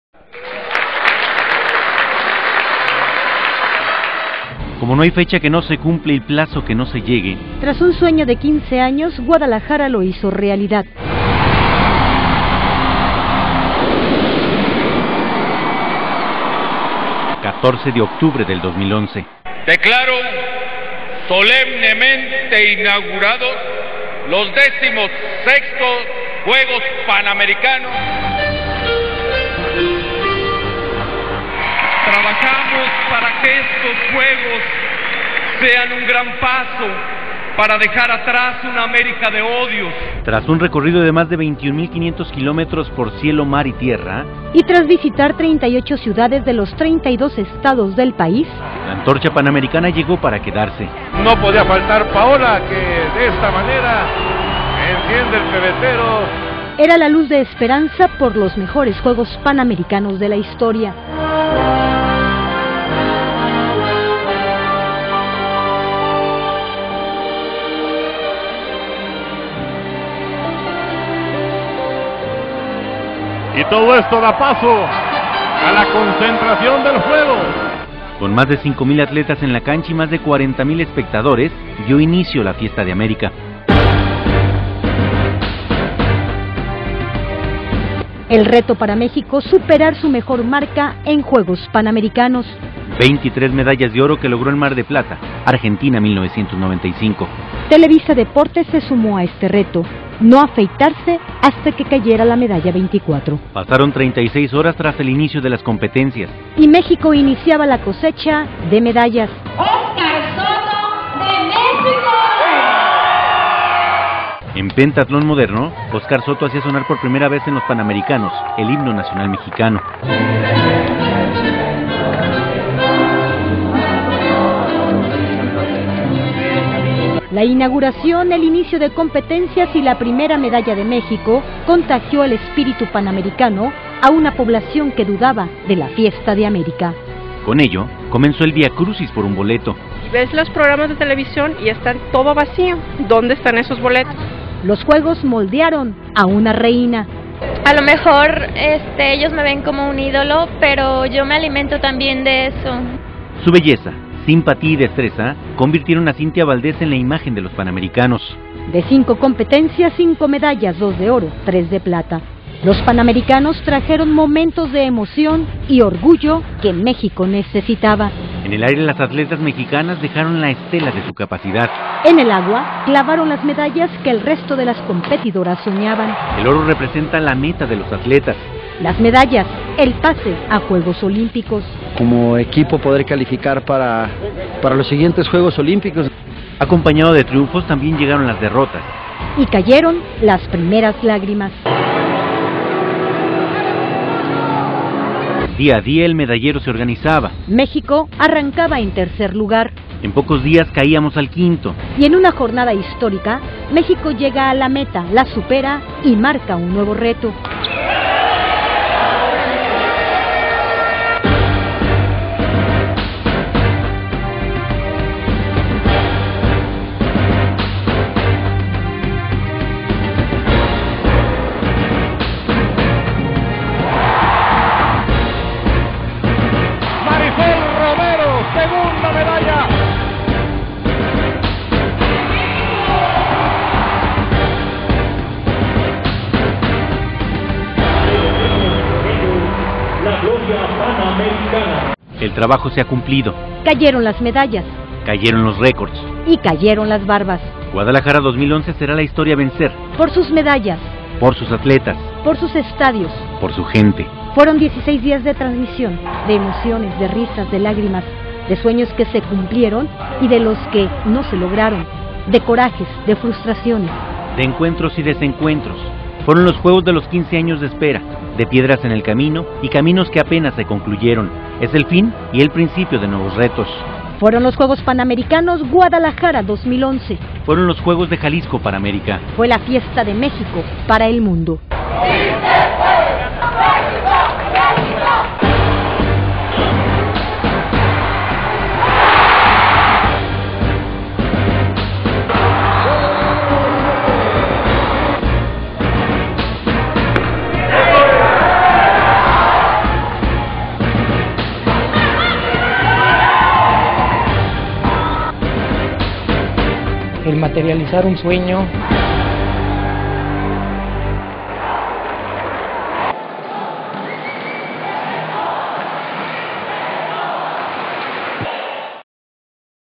Juegos Panamericanos 2011
Crónica de lo sucedido con las voces de Ernesto Villalpando y Becky Reynoso durante los juegos panamericanos 2011 en Televisa Guadalajara